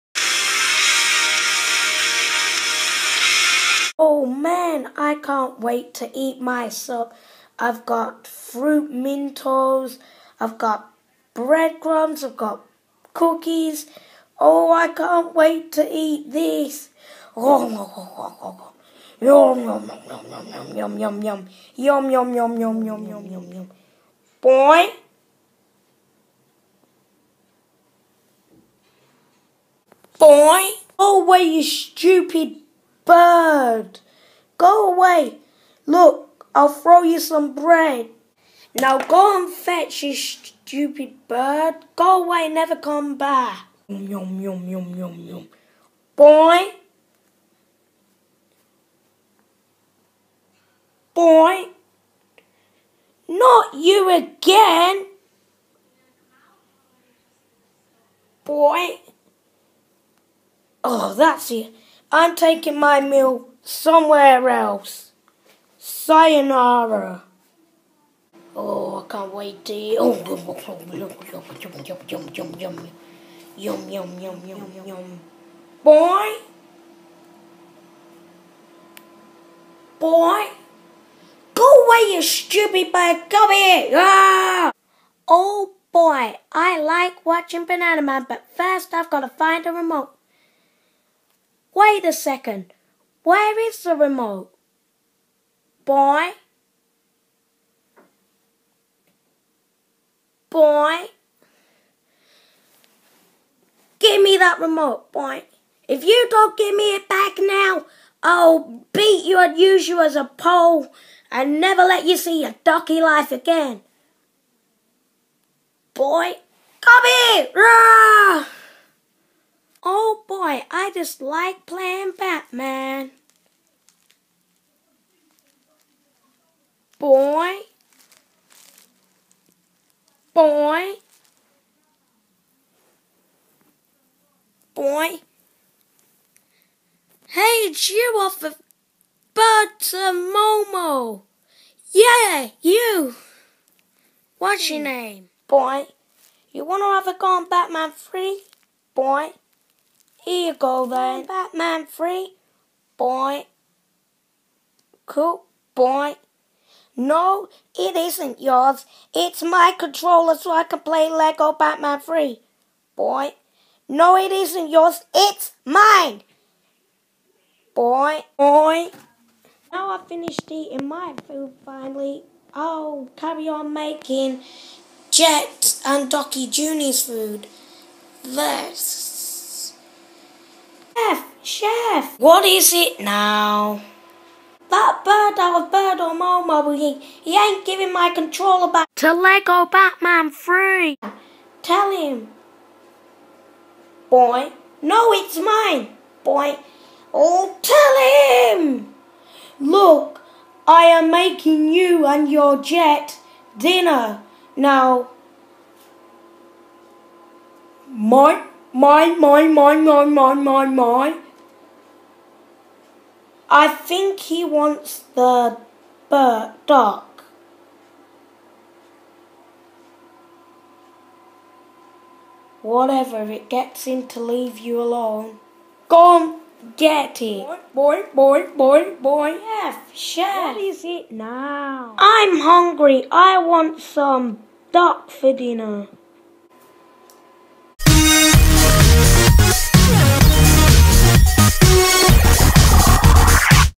GHH: Ducky Junior season 1 episode 2 The thief
Every one is happy but there is some one takin Every bodes stuff